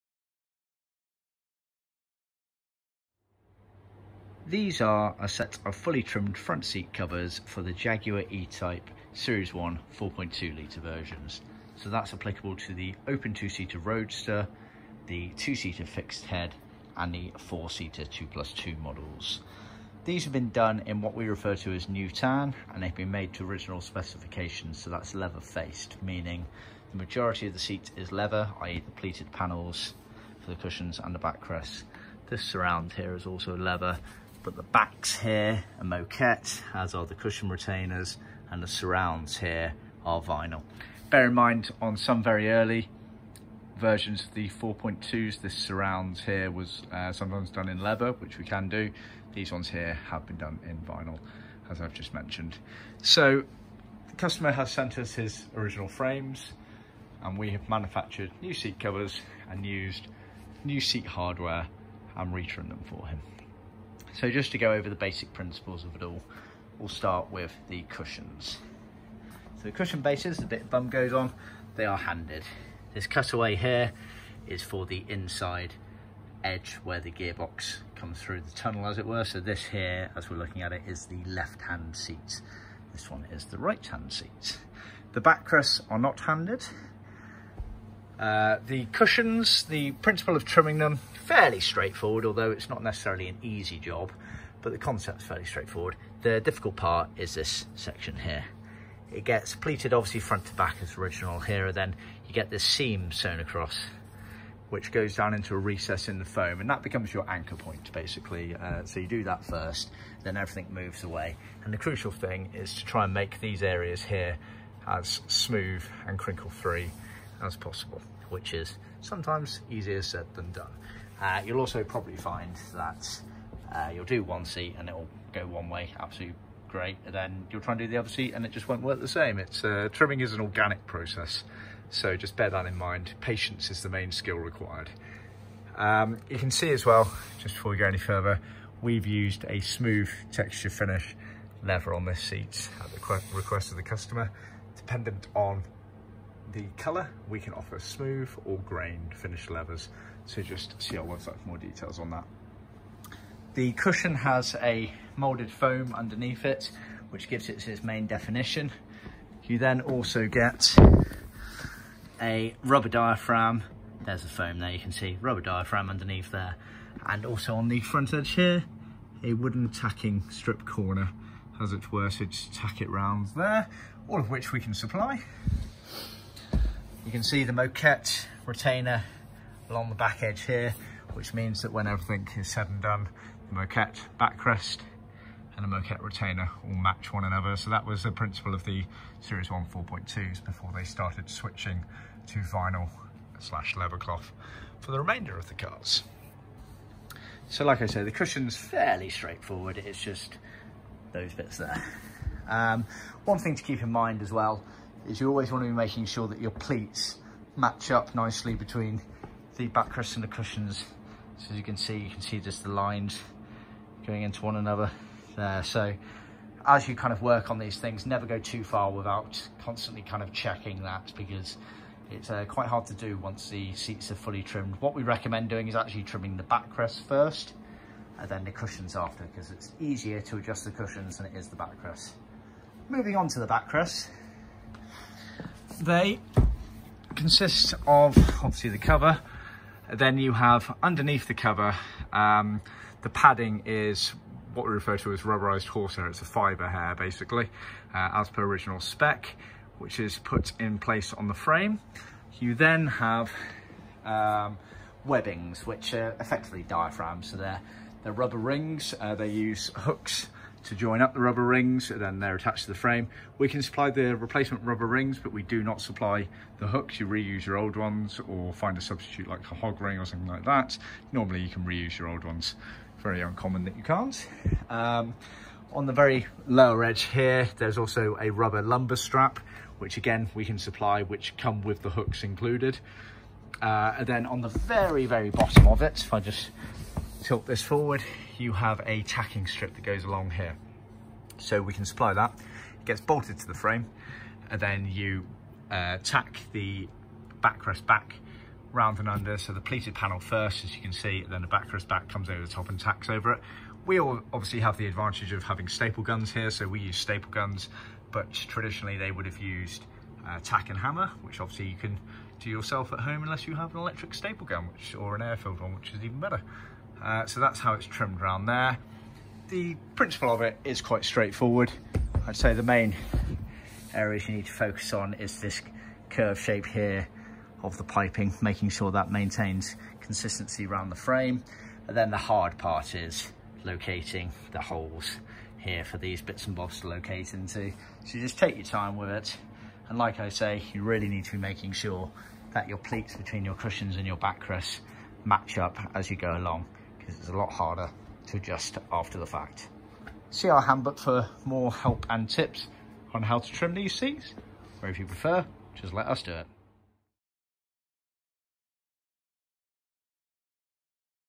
Jaguar "E" Type - Trimmed Front Seats (S1, 4.2ltr)
A short video to show a set of fully trimmed Front Seat Seats for the 4.2ltr Jaguar E Type (XK-E) models - applicable to the Open Two-Seater (OTS) Roadsters, 2-Seater Fixed Head Coupé (FHC), as well as the 4 Seater Two-plus-Two (2+2) cars.

These have been trimmed by John Skinner Ltd using our new Seat Covers, Foams, Fibres, Tacking Strips, Lumbers Straps, Diaphragms and Webbings. The Seat Frames are the originals supplied by our client.

Suitable Models: E Type (XK-E) 4.2ltr Series 1
Materials: Leather "Faced" (Leather Facings with Vinyl and Moquette elements)

© John Skinner (Manufacturing) Ltd (2022)